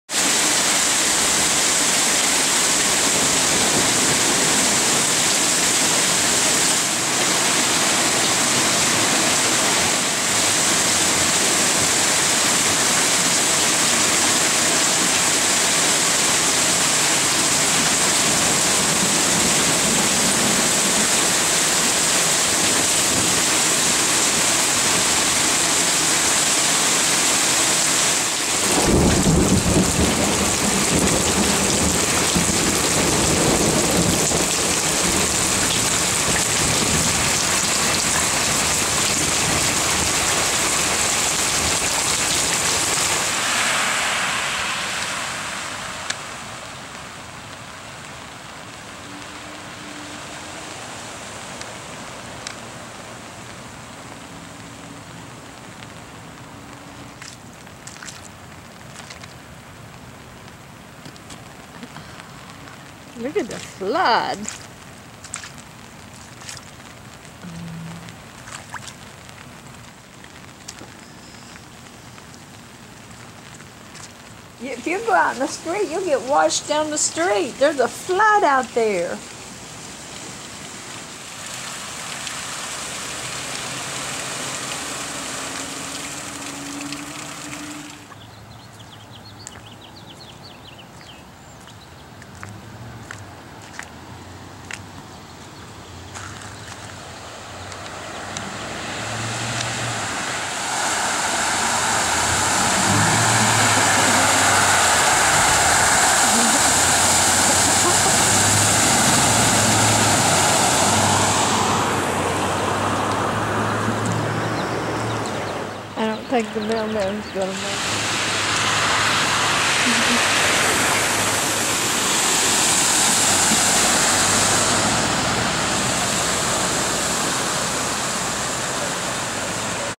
Sun City, Az Storm
A wild rainstorm that dropped a lot of rain in a hurry on Sun City, 3-1-14.  We received an inch and a half within a few hours.